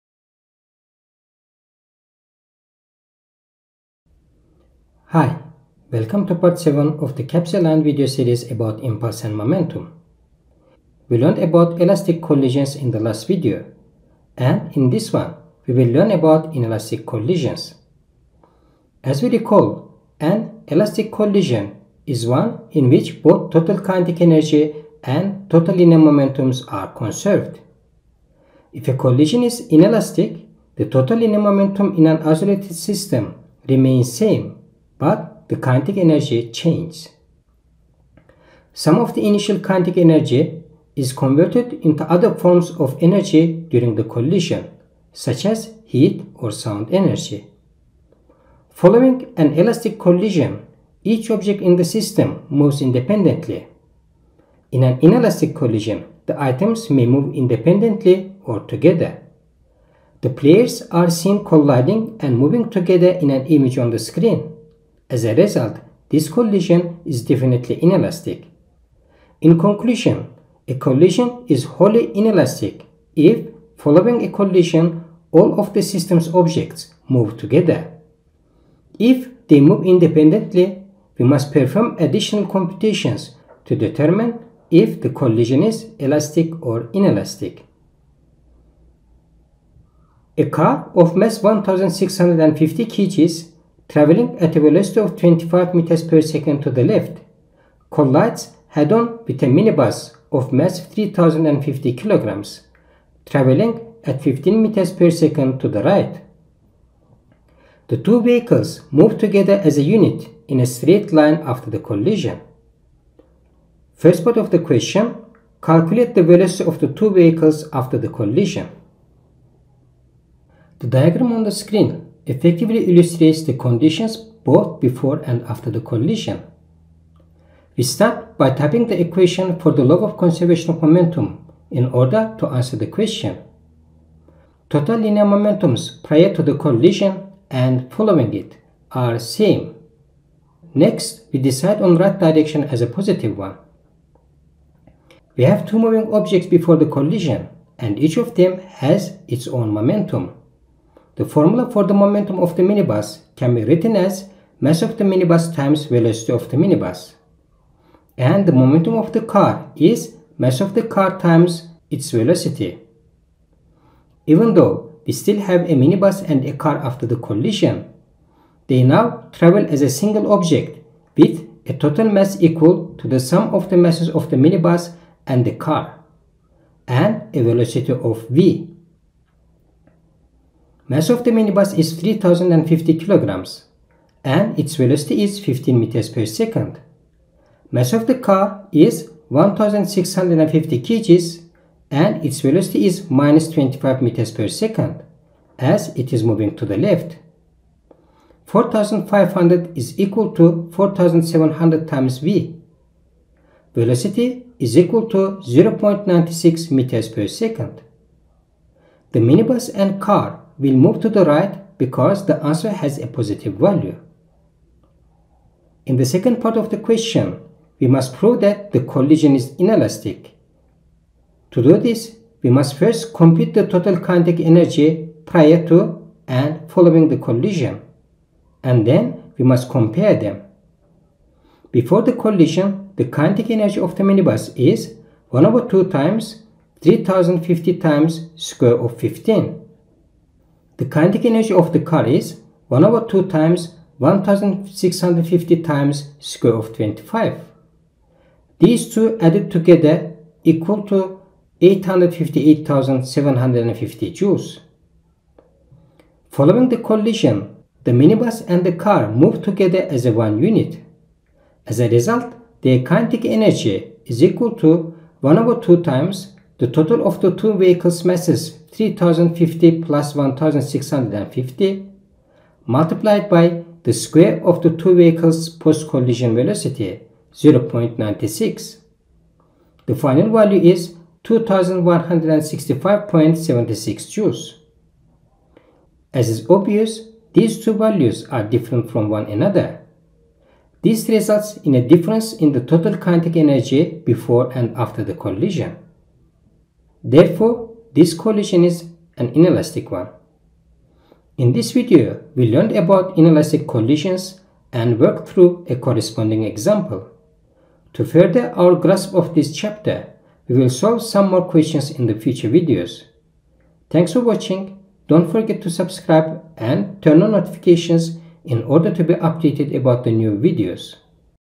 Physical Sciences - Gr 12 - Impulse and Momentum - (Part 07 - Inelastic Collision)
Welcome to part 7 of the CAPS aligned video series about Impulse and Momentum.
We learned about elastic collisions in the last video, and in this one, we'll learn about inelastic collisions.

TIMESTAMPS
00:00 Introduction
00:28 Definition of an Inelastic Collision
01:28 Example - Part 1
03:34 Example - Part 2